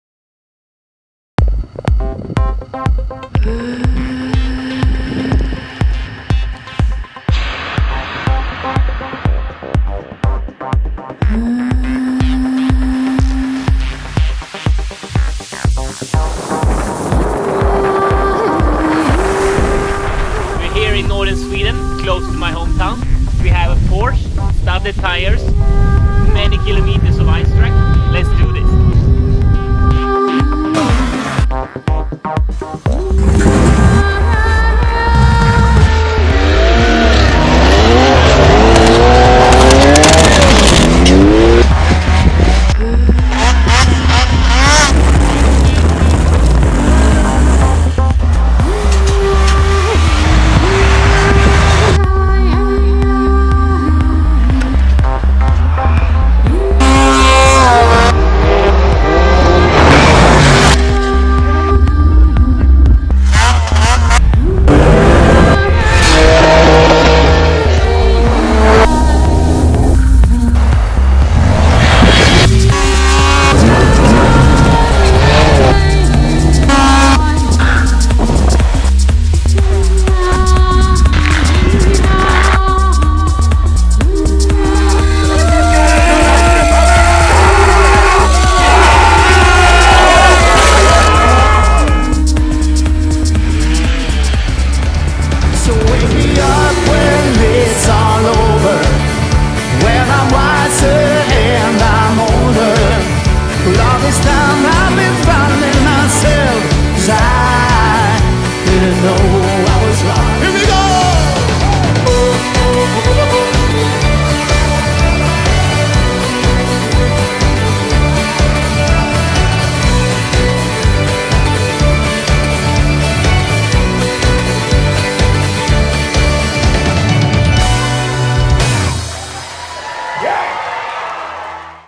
FlatOut Sweden 2015 Trailer
FlatOut Sweden is a company that provides you with the most extreme combinations of winter activities in Northern Sweden. Most of the events start or end in the ski resort Åre. / Patrik Sandell CEO